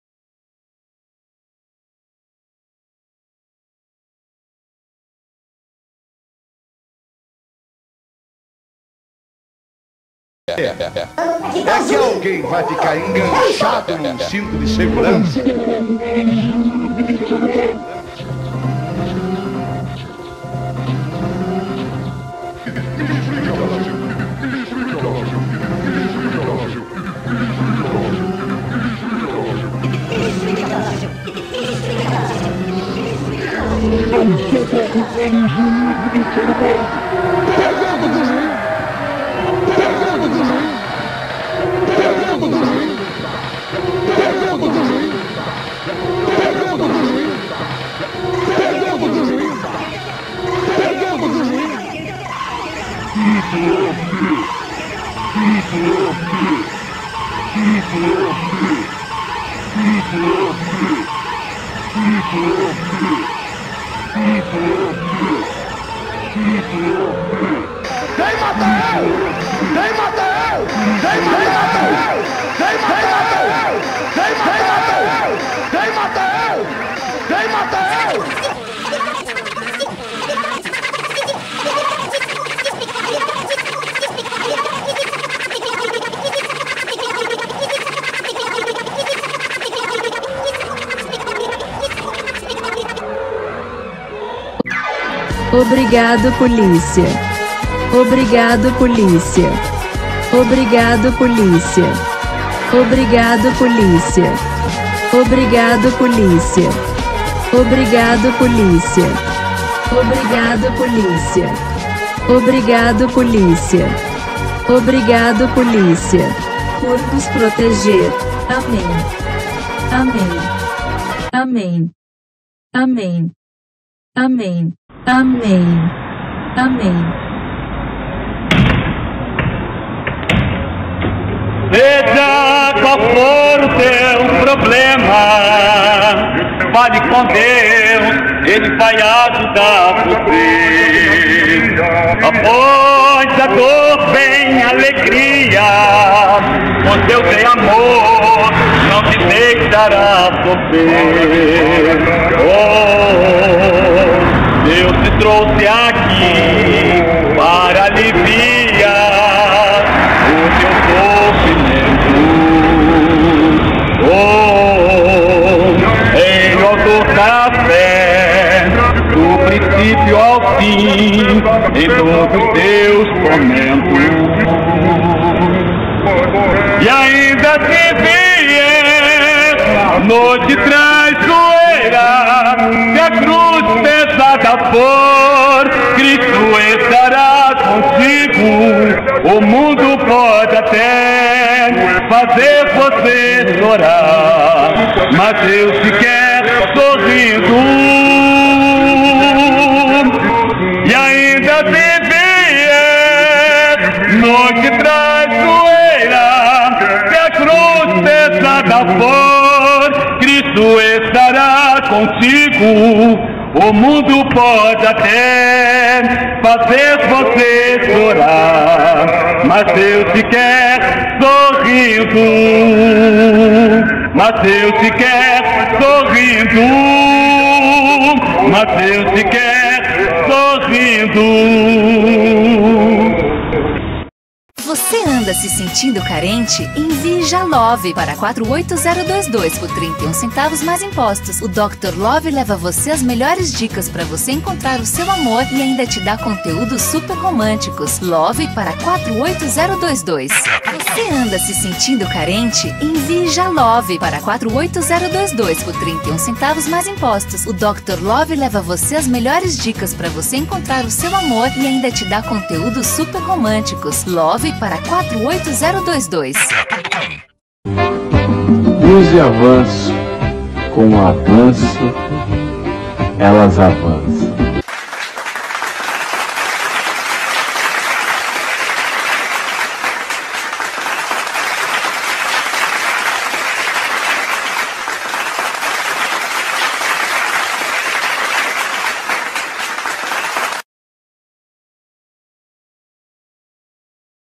OBRIGADO POLÍCIA! (2021)
"A polícia salva o Brasil do desastre e do comunismo"
Por Vinícius Veloso